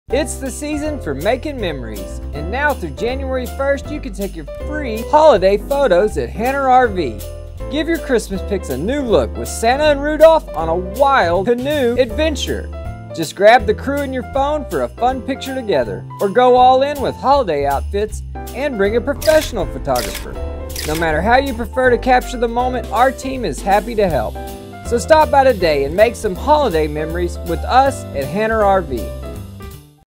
Make some Holiday Memories with us at Hanner RV!
Come take your FREE Holiday Photos with Hanner RV. Now through January first, stop by anytime we're open to take your family and friends on a wild canoe adventure! You can go all in with holiday outfits and a professional photographer to capture the moment or just snap a few quick pics with your phone! No matter how you prefer to capture the moment, our Hanner RV is happy to help.